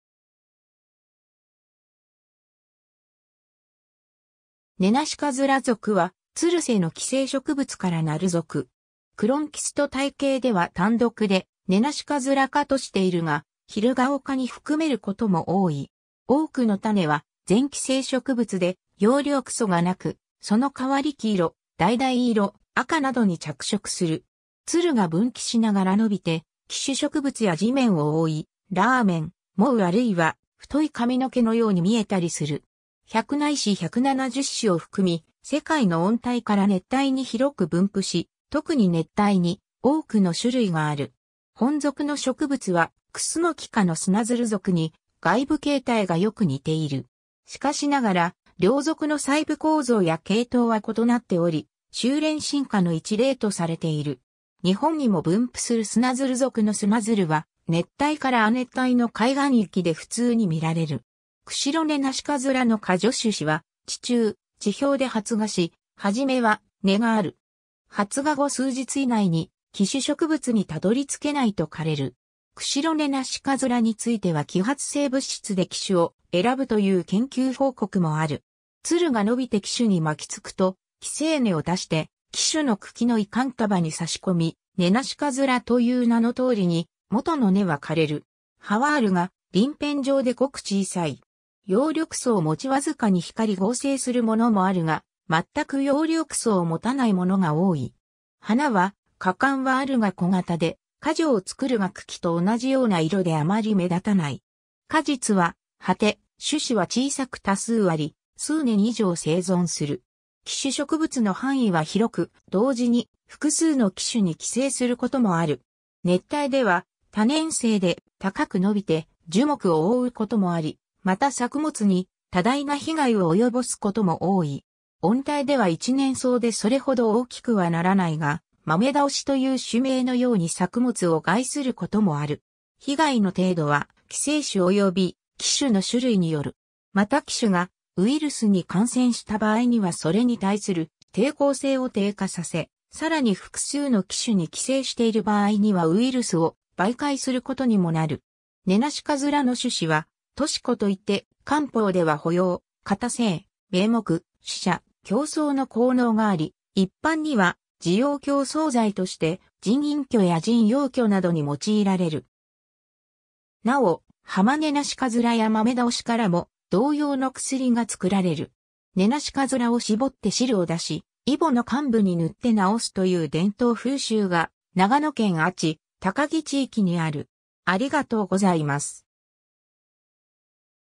ネナシカズラ属
ネナシカズラ属（ネナシカズラぞく、Cuscuta）はつる性の寄生植物からなる属。
クロンキスト体系では単独でネナシカズラ科 (Cuscutaceae) としているが、ヒルガオ科に含めることも多い（分子遺伝学的にもヒルガオ科の系統に属すとされるので、APG植物分類体系でもヒルガオ科とする）。
 多くの種は全寄生植物で葉緑素がなく、その代わり黄色、橙色、赤などに着色する。
つるが分岐しながら伸びて寄主植物や地面を覆い、「ラーメン」・「網」あるいは「太い髪の毛」のように見えたりする。
 100ないし170種を含み、世界の温帯から熱帯に広く分布し、特に熱帯に多くの種類がある。
 本属の植物はクスノキ科のスナヅル属に外部形態がよく似ている。
しかしながら、両属の細部構造や系統は異なっており、収斂進化の一例とされている。
日本にも分布するスナヅル属のスナヅルは熱帯から亜熱帯の海岸域で普通に見られる。
 クシロネナシカズラの花序 種子は地中・地表で発芽し、初めは根がある。
発芽後数日以内に寄主植物にたどり着けないと枯れる。
クシロネナシカズラについては揮発性物質（"におい"）で寄主を選ぶという研究報告もある。
つるが伸びて寄主に巻きつくと、寄生根を出して寄主の茎の維管束に挿しこみ、ネナシカズラという名の通りに元の根は枯れる。
 葉はあるが鱗片状でごく小さい。
葉緑素を持ちわずかに光合成するもの（C. reflexaなど）もあるが、全く葉緑素を持たないものが多い。
花は花冠はある（つぼ状、5裂）が小型で、花序をつくるが茎と同じような色であまり目立たない。
果実は蒴果、種子は小さく多数あり、数年以上生存する。
 寄主植物の範囲は広く、同時に複数の寄主に寄生することもある。
熱帯では多年生で、高く伸びて樹木を覆うこともあり、また作物に多大な被害を及ぼすことも多い。
温帯では一年草でそれほど大きくはならないが、マメダオシという種名のように作物を害することもある。
被害の程度は寄生種および寄主の種類による。
また寄主がウイルスに感染した場合にはそれに対する抵抗性を低下させ、さらに複数の寄主に寄生している場合にはウイルスを媒介することにもなる。
 ネナシカズラの種子は菟絲子（としし、菟糸子とも）といって漢方では補陽・固精・明目・止瀉・強壮の効能があり、一般には滋養強壮剤として腎陰虚や腎陽虚などに用いられる。
なお、ハマネナシカズラやマメダオシからも同様の薬が作られる。
 ネナシカズラを搾って汁を出し、イボの患部に塗って治すという伝統風習が長野県阿智・喬木地域にある。